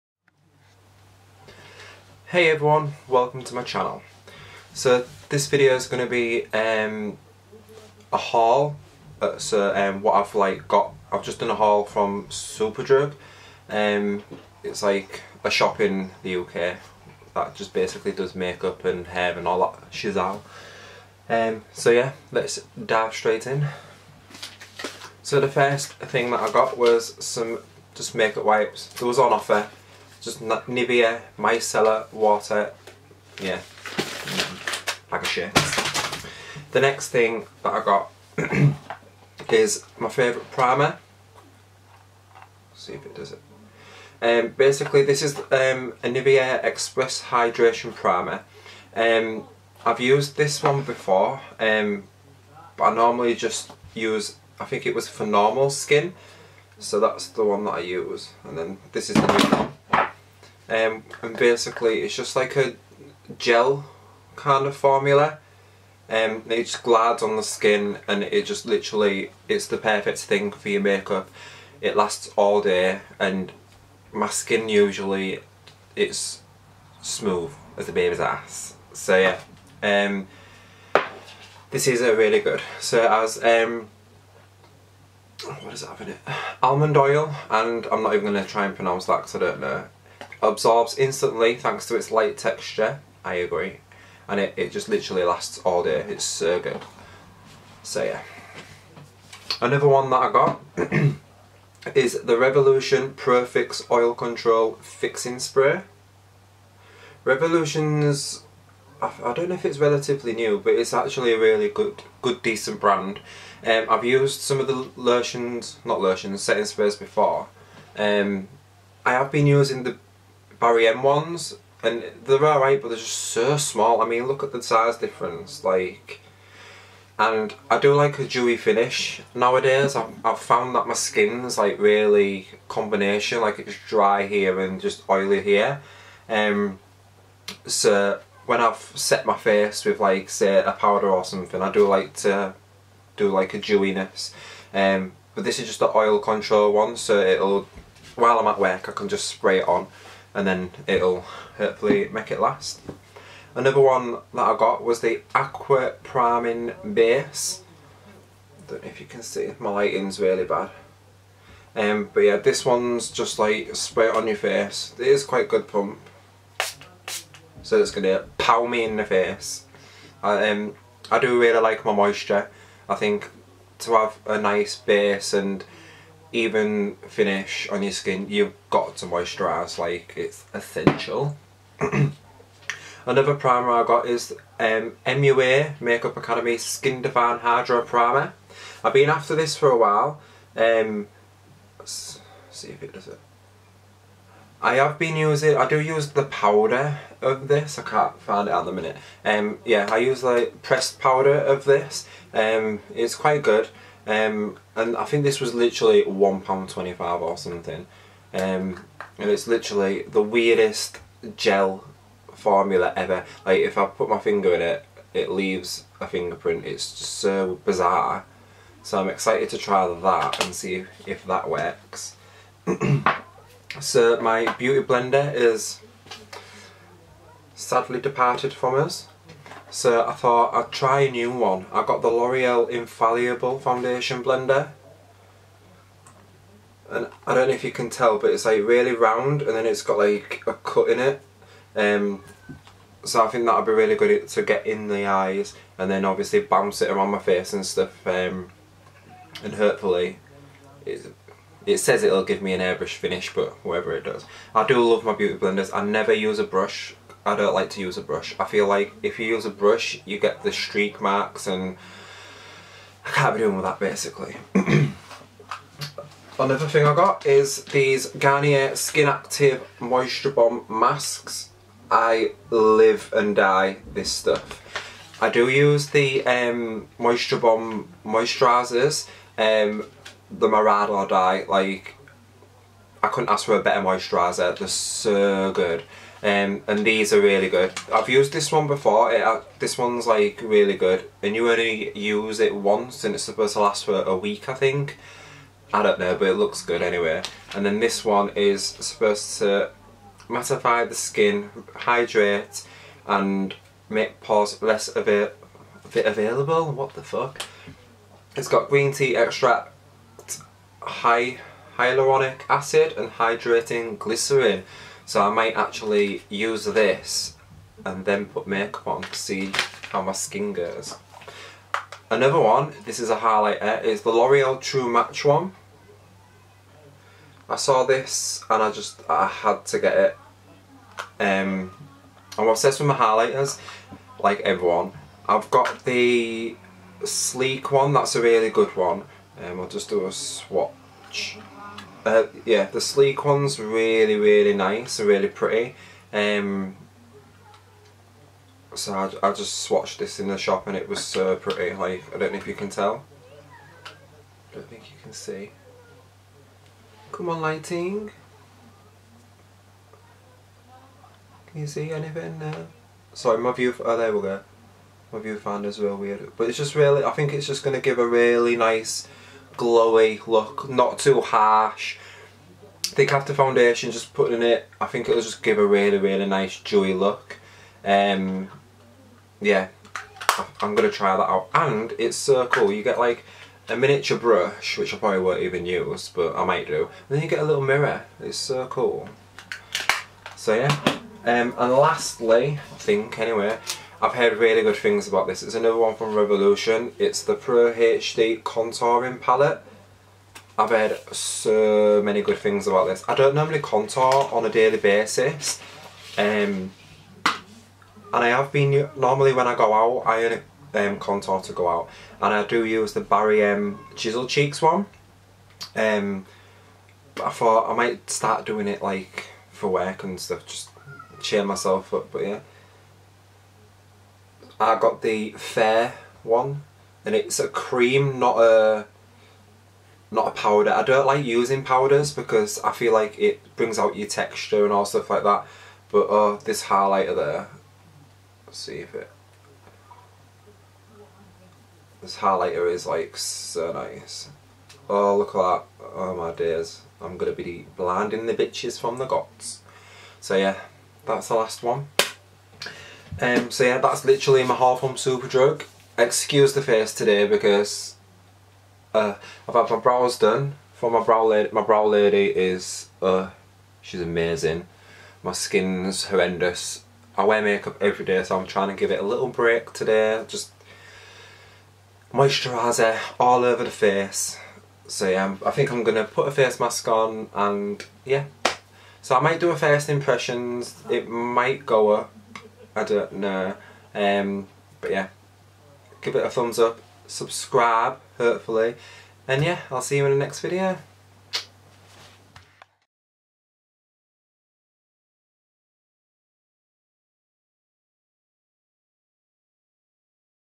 Superdrug Makeup Haul / First Video
hey guys! this is my first ever video! be nice!
just a little haul i got today from the drugstore, what better way to introduce myself to youtube than looking like a mess and doing this haha!